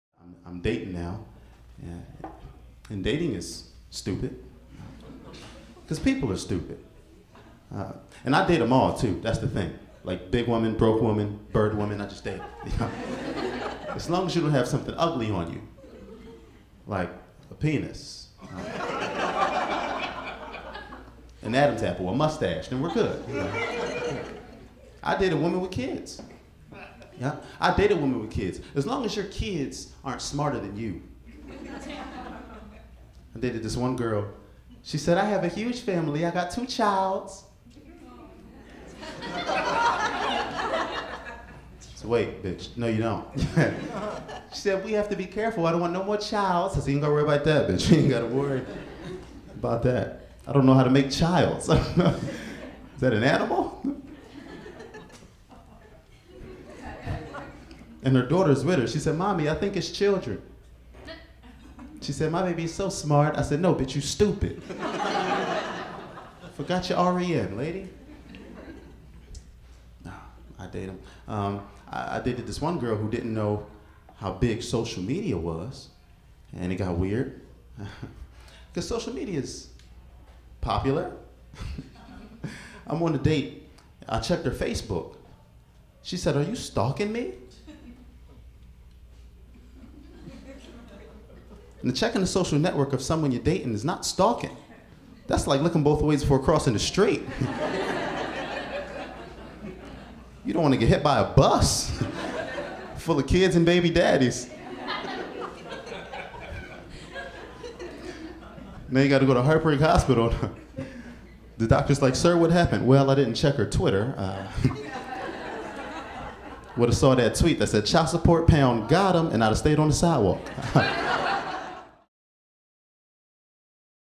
Erik Terrell: Dating Problems (Stand Up Comedy)
Erik Terrell will date just about anyone.